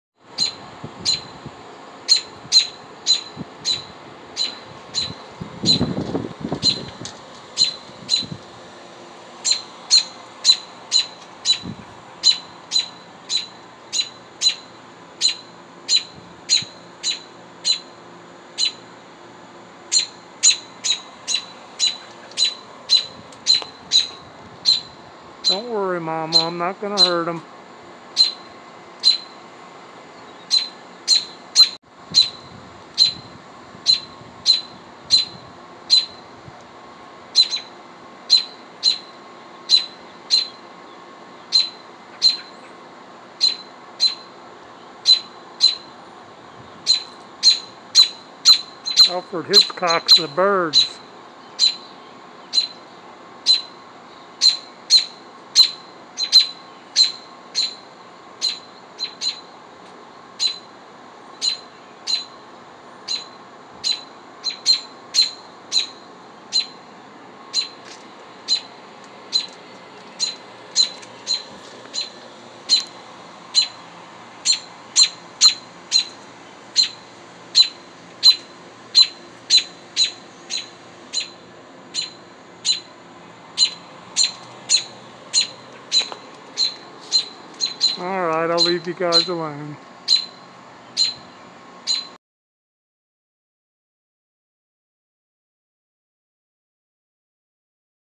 Family of Robins
I found a Robins nest at work, Momma kept a close eye on me.